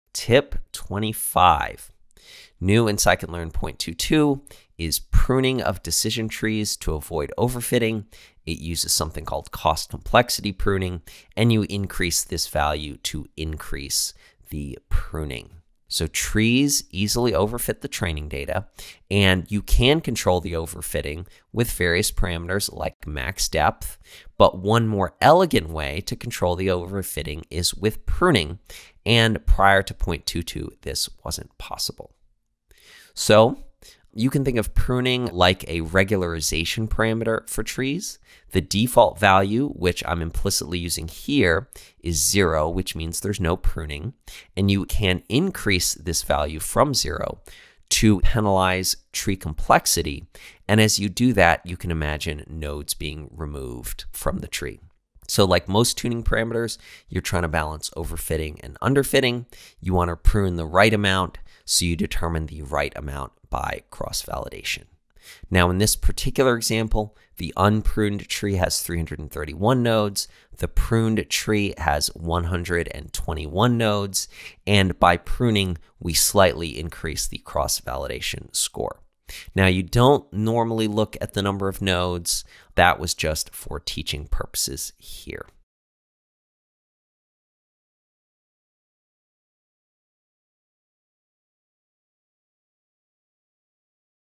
Prune a decision tree to avoid overfitting
New in scikit-learn 0.22: Pruning of decision trees to avoid overfitting!
- Uses cost-complexity pruning
- Increase "ccp_alpha" to increase pruning (default value is 0)

👉 New tips every TUESDAY and THURSDAY! 👈

=== WANT TO GET BETTER AT MACHINE LEARNING? ===